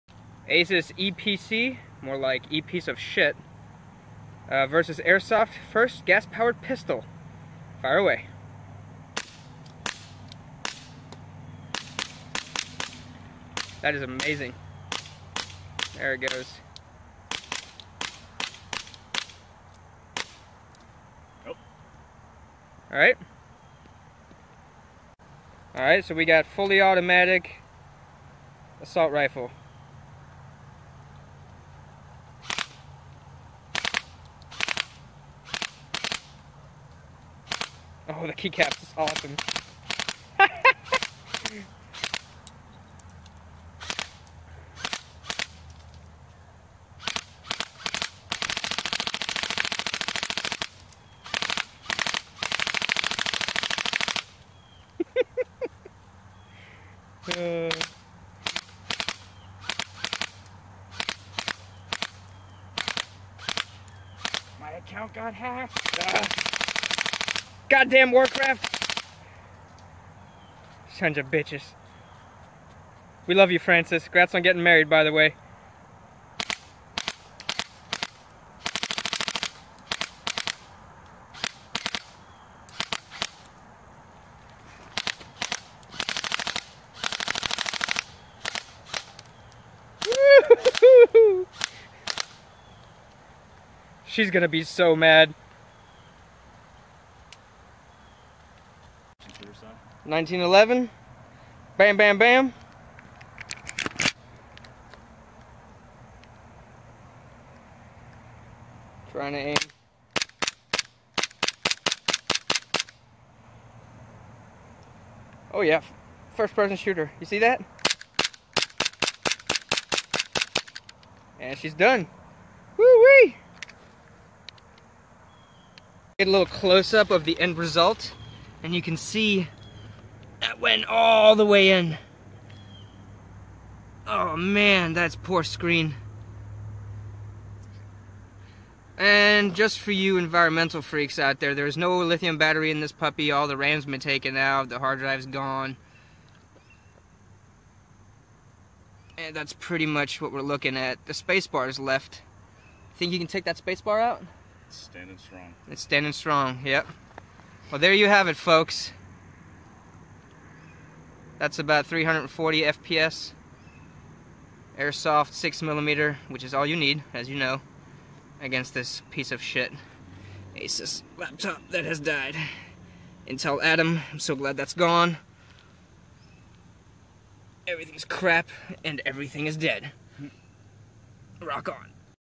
Asus eePC vs Airsoft.
Round 1 was a green gas powered pistol. Round 2 was a fully automatic AEG. Round 3 was my green gas powered 1911. Some of the sound got messed up from echoes or something. More to come!